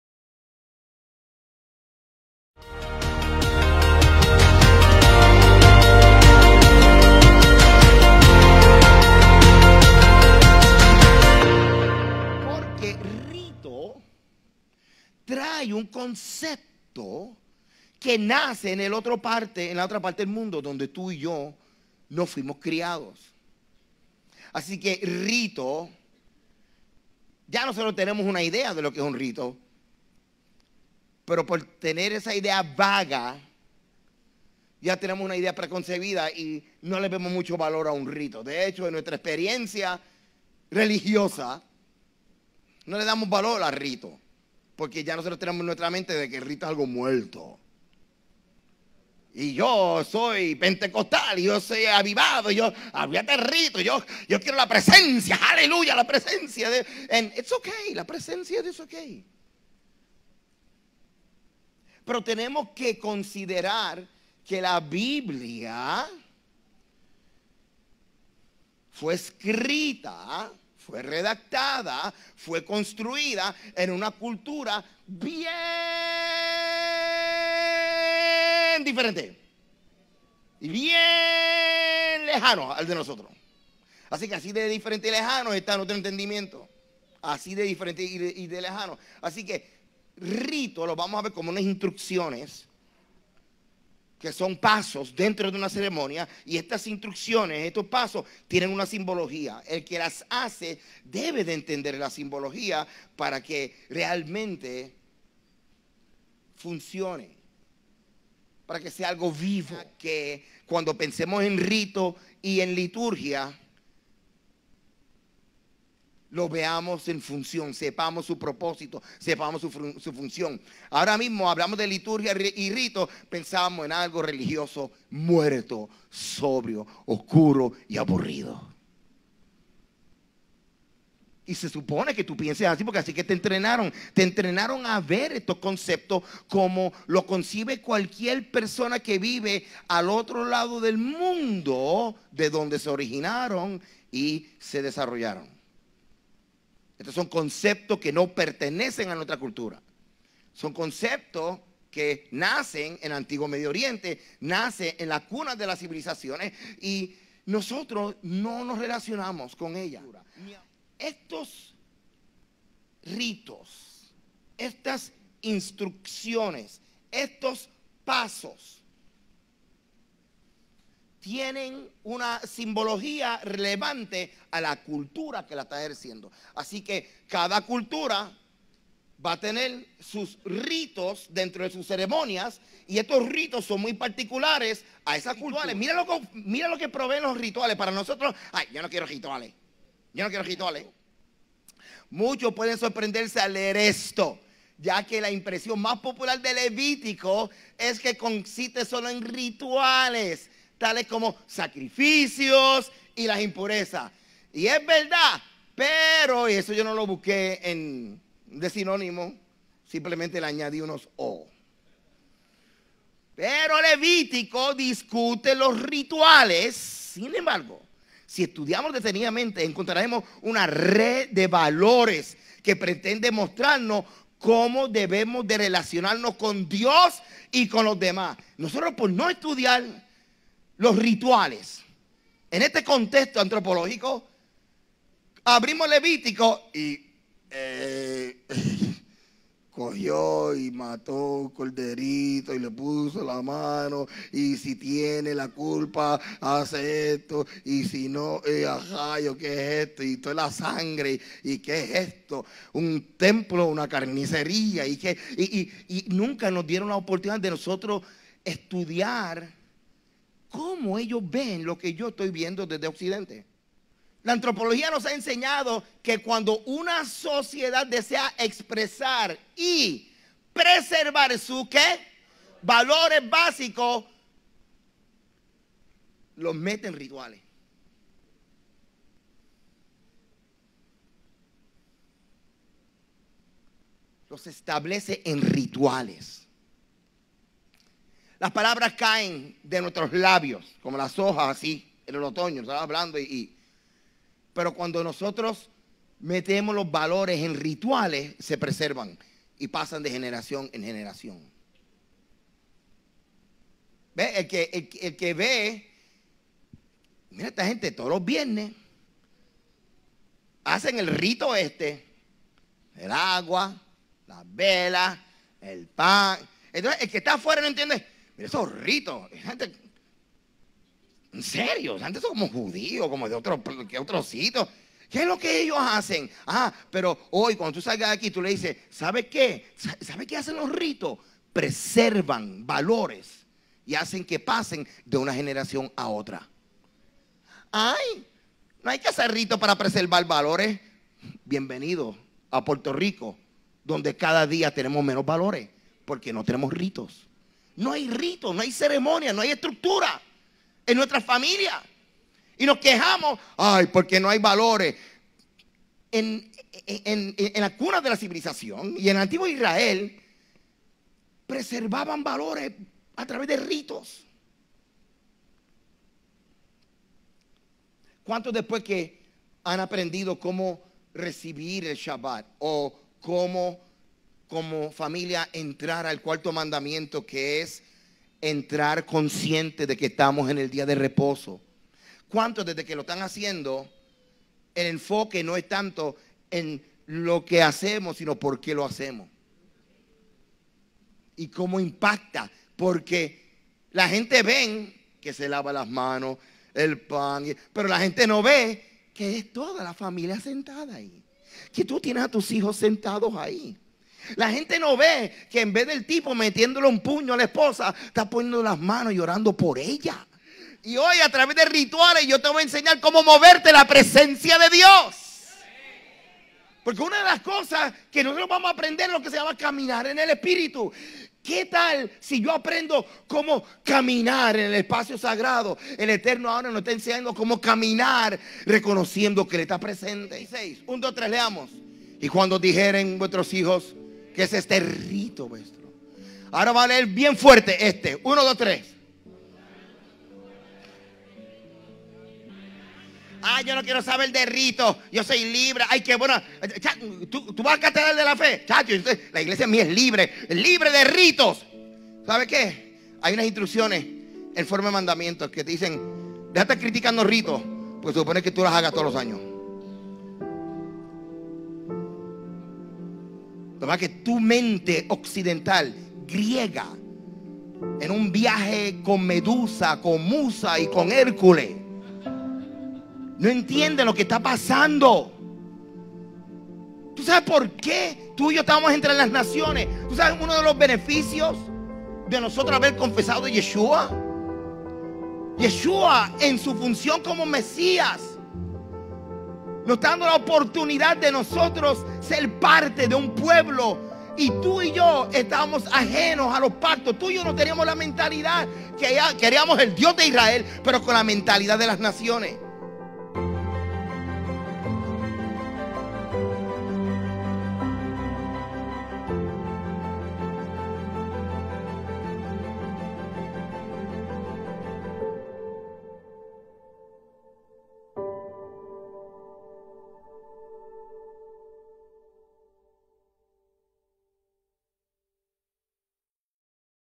"Ritos, liturgia y la presencia de Dios." (Porción de Vayikra 2019)
Registrate gratis en nuestra pagina web

Por no darnos la oportunidad de estudiar el contexto cultural / antropológico de conceptos en la biblia, nos hemos cohibido de sumergirnos en profundidades practicas y sumamente espirituales que enriquecerán nuestra relación con Dios. Concesptos culturales que encontramos en las escritura NO SE PUEDEN PROCESAR con nuestra mente occidental, tenemos que estirarnos es ESTUDIAR. Uno de los grandes retos es relacionar Ritos, liturgia y el honor de acercarnos a la presencia de Dios. Si no estudiamos también impediremos que un pueblo que anhela mas de la presencia de Dios tenga tal honor y experiencia transformadora.
Aquí solo una "Ritos, liturgia y la presencia de Dios.". Espero que les sea de bendición.